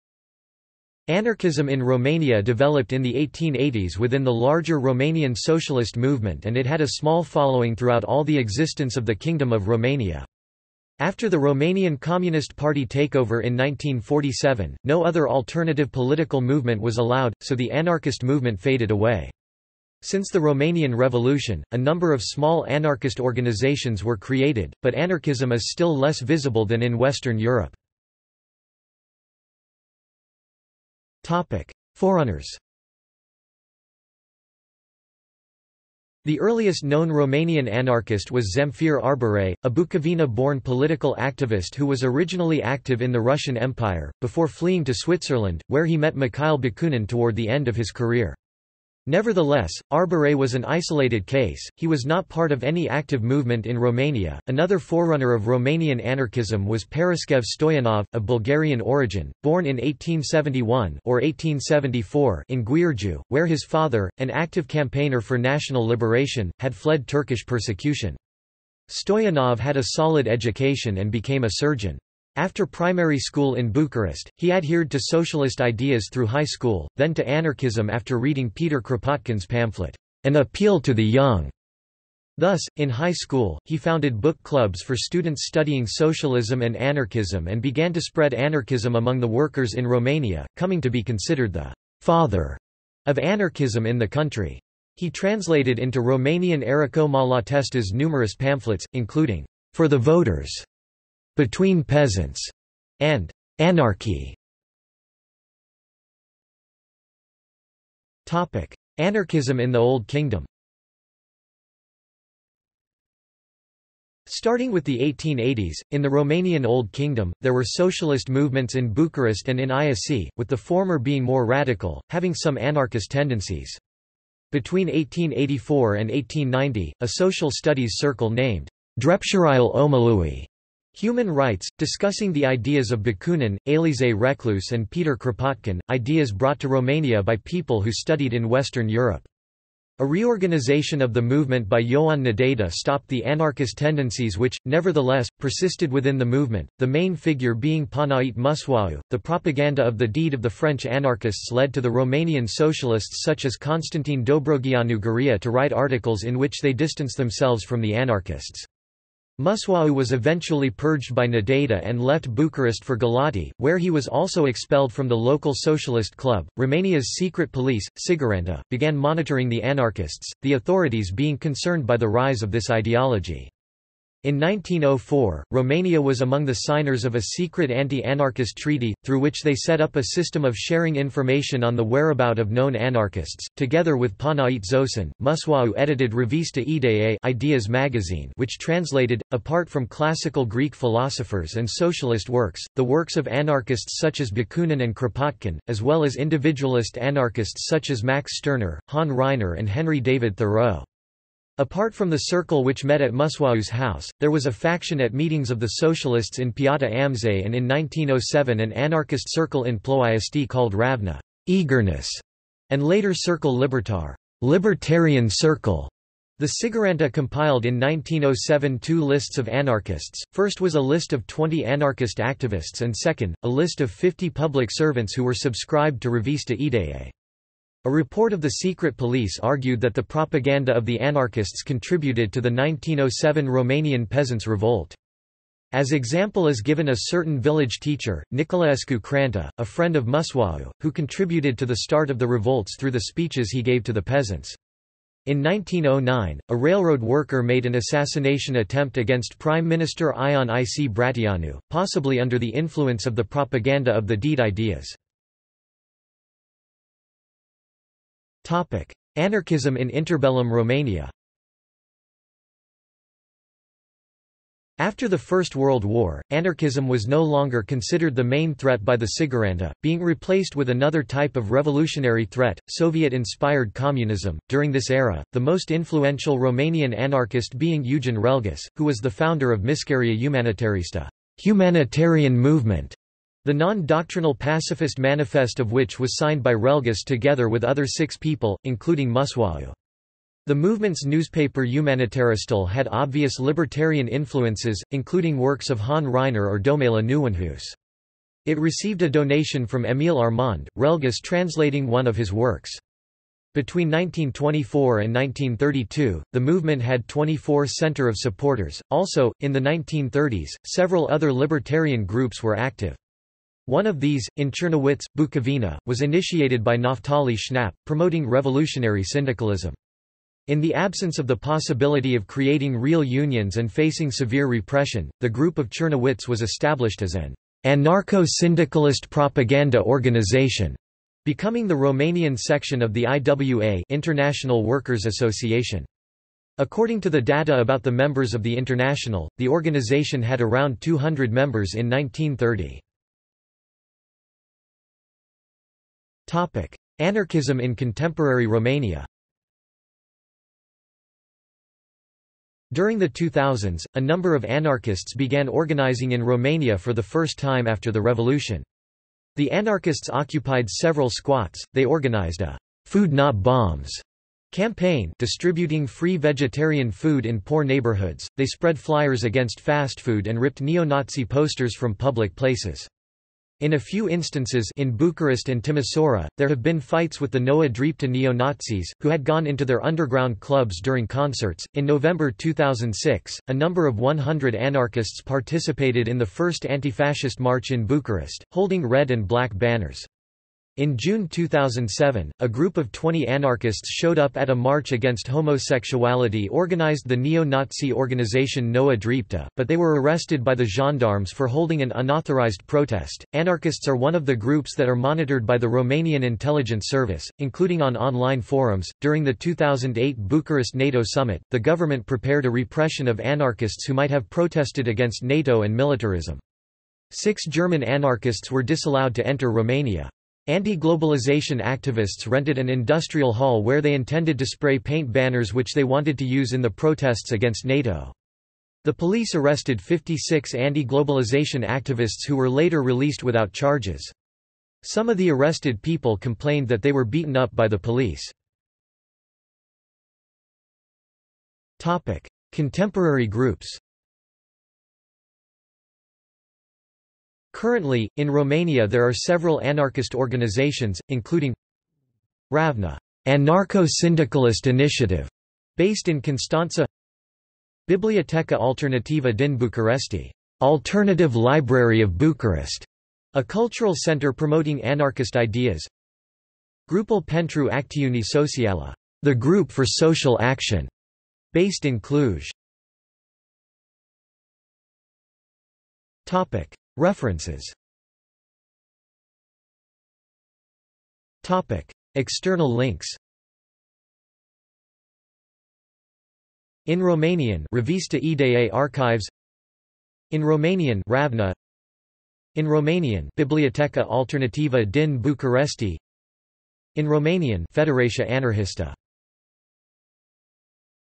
Anarchism in Romania | Wikipedia audio article
This is an audio version of the Wikipedia Article:
Anarchism in Romania

- Socrates

SUMMARY
Anarchism in Romania developed in the 1880s within the larger Romanian socialist movement and it had a small following throughout all the existence of the Kingdom of Romania. After the Romanian Communist Party takeover in 1947, no other alternative political movement was allowed, so the anarchist movement faded away. Since the Romanian Revolution, a number of small anarchist organizations were created, but anarchism is still less visible than in Western Europe.